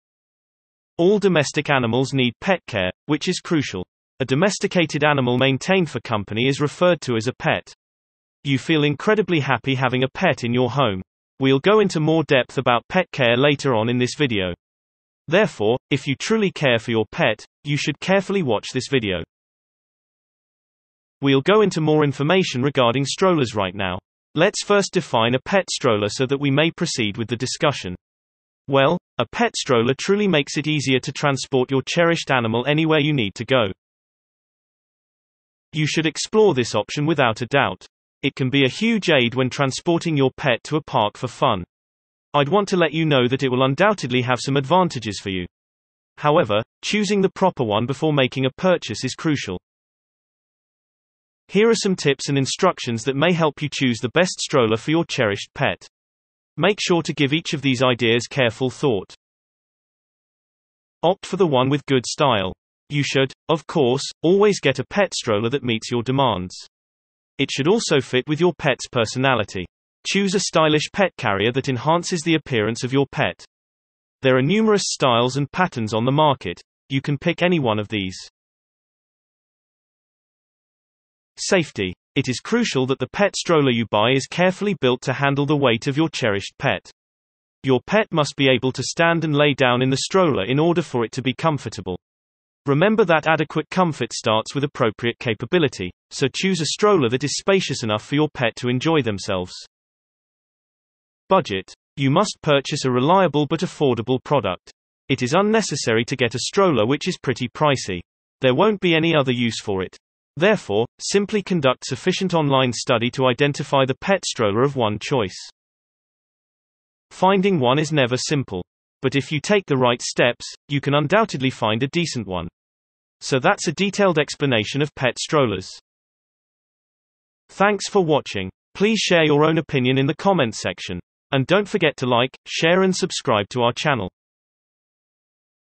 Why you should go for the best stroller#Bestcatstrollers
If you are looking for a cat stroller that will make life easier for you and your cat, the best option is to go for a cat stroller that has a lot of features. A good cat stroller should have a lot of storage space, a comfortable seat, and a canopy that will protect your cat from the sun and rain. Here are some reasons why you should go for the best cat stroller:
A good cat stroller will have a lot of storage space. You can store all of your cat's supplies, such as her food, water dishes, and toys, in the stroller. This will make traveling with your cat easier.